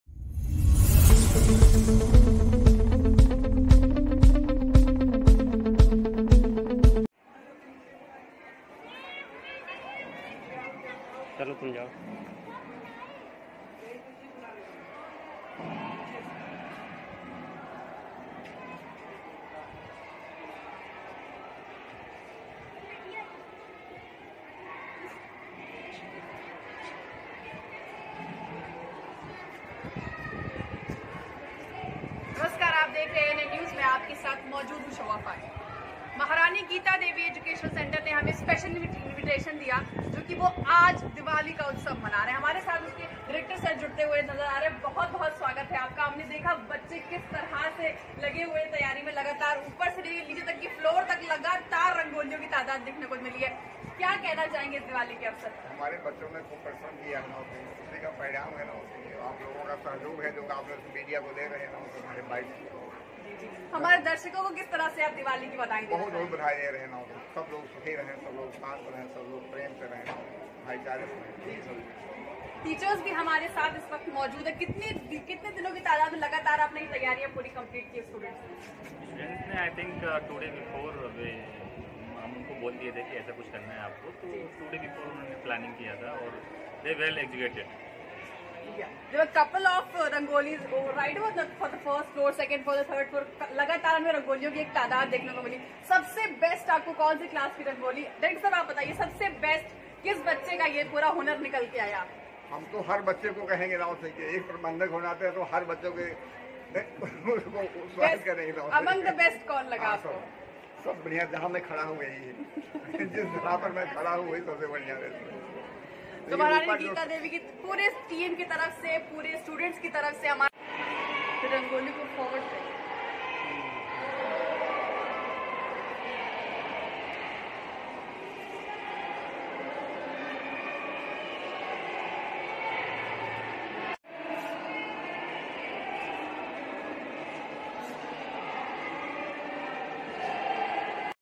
दीपावली के पावन पर्व पर दीप एवं रंगोली प्रतियोगिता का आयोजन
महारानी गीता देवी एजुकेशन सेंटर में दीपावली के पावन पर्व पर कराई गई दीप एवं रंगोली प्रतियोगिता का आयोजन